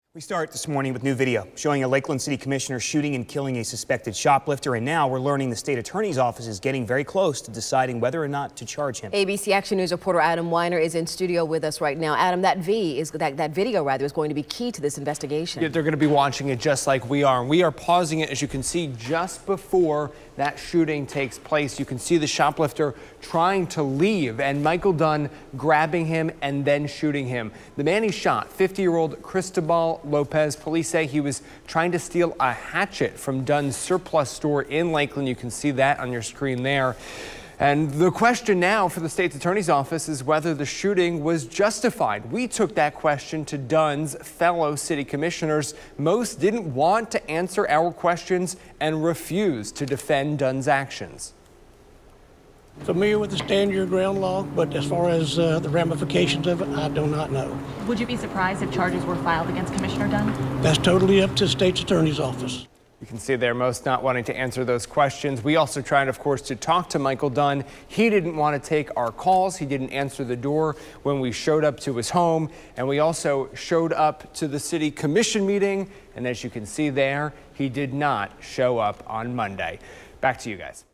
Police release video of commissioner shooting
Lakeland Police released surveillance video which shows a Lakeland City Commissioner shoot and killed a man who tried to steal a hatchet from his military surplus store.